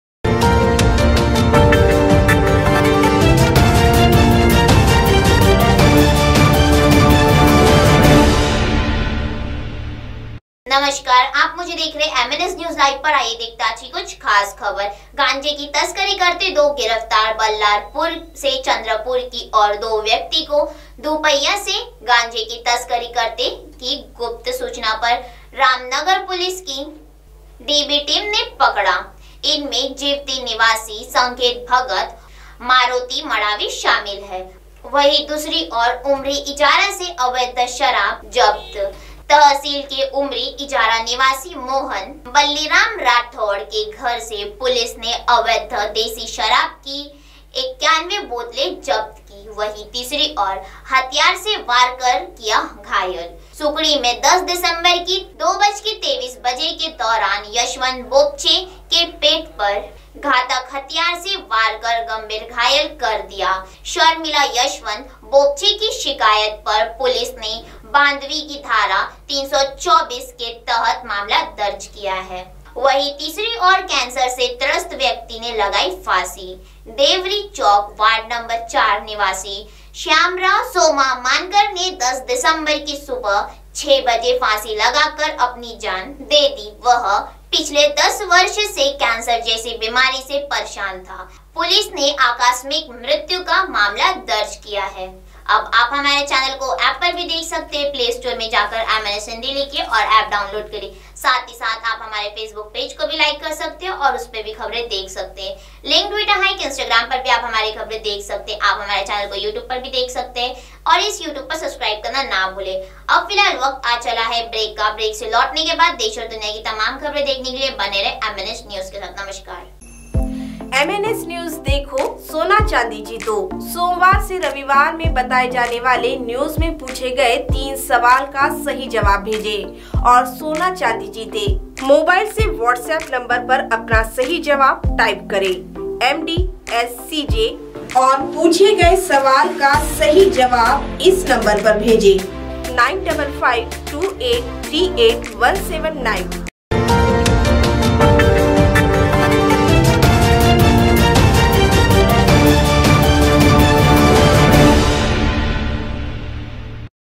गांजा की तस्करी करते २ गिरफ्तार
चंद्रपुर : गांजा  की तस्करी करते २ गिरफ्तार |